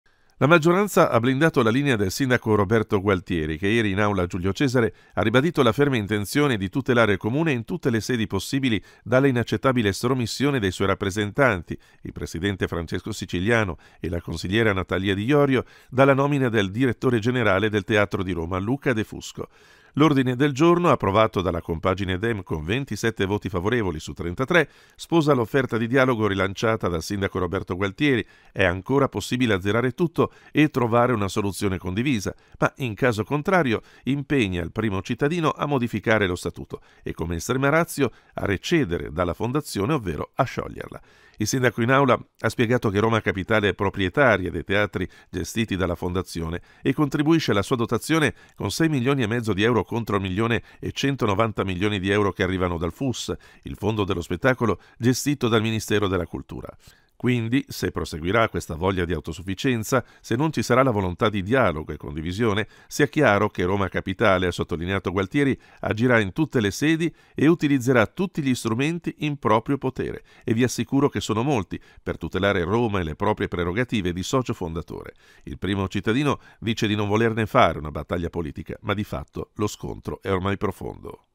Teatro di Roma, Comune pronto a sciogliere la Fondazione
La maggioranza ha blindato la linea del sindaco Roberto Gualtieri, che ieri in aula Giulio Cesare ha ribadito la ferma intenzione di tutelare il Comune “in tutte le sedi possibili” dalla “inaccettabile estromissione” dei suoi rappresentanti (il presidente Francesco Siciliano e la consigliera Natalia Di Iorio) dalla nomina del dg del Teatro di Roma, Luca De Fusco. L’ordine del giorno approvato dalla compagine dem con 27 voti favorevoli su 33 sposa l’offerta di dialogo rilanciata dal sindaco Roberto Gualtieri («È ancora possibile azzerare tutto e trovare una soluzione condivisa»), ma in caso contrario impegna il primo cittadino a modificare lo statuto e - come extrema ratio - a «recedere dalla Fondazione» ovvero a «scioglierla». Il sindaco in aula ha spiegato che Roma Capitale è proprietaria dei teatri gestiti dalla Fondazione e contribuisce alla sua dotazione con 6,5 milioni di euro contro il milione e 190 milioni di euro che arrivano dal Fus, il fondo dello spettacolo gestito dal ministero della Cultura. Quindi «se proseguirà questa voglia di autosufficienza, se non ci sarà la volontà di dialogo e condivisione, sia chiaro che Roma Capitale – ha sottolineato Gualtieri - agirà in tutte le sedi e utilizzerà tutti gli strumenti in proprio potere, e vi assicuro che sono molti, per tutelare Roma e le proprie prerogative di socio fondatore». Il primo cittadino dice di non volerne fare una battaglia politica ma lo scontro è ormai profondo.